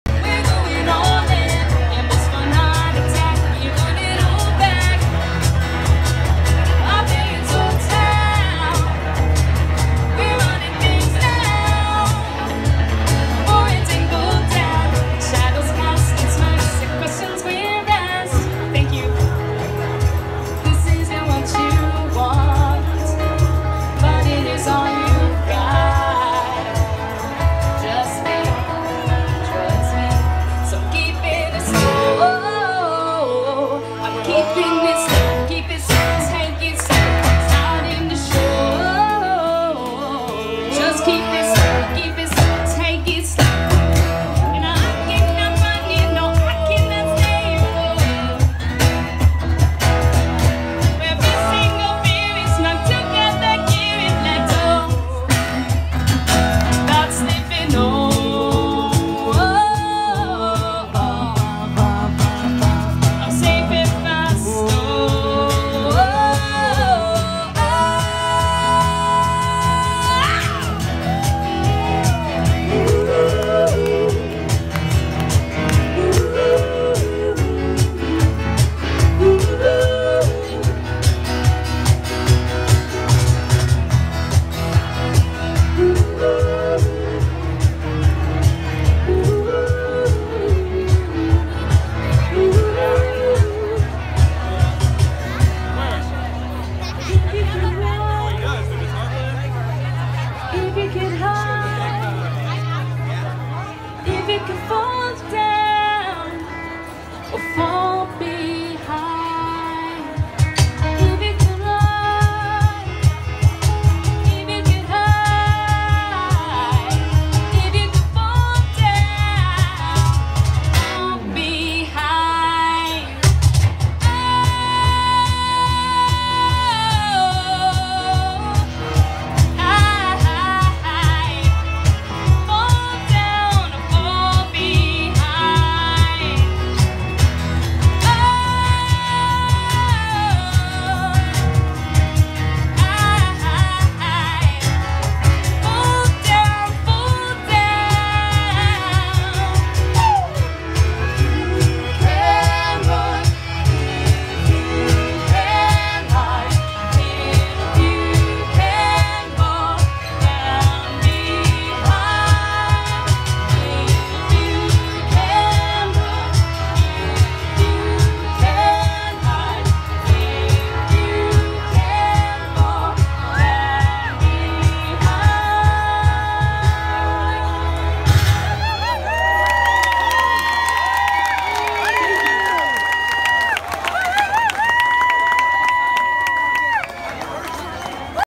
Sanford Florida 4th Annual Porchfest - Cat Ridgeway & The Tourists
Cat Ridgeway & The Tourists on Porch #7 February 27, 2021
Music not my property.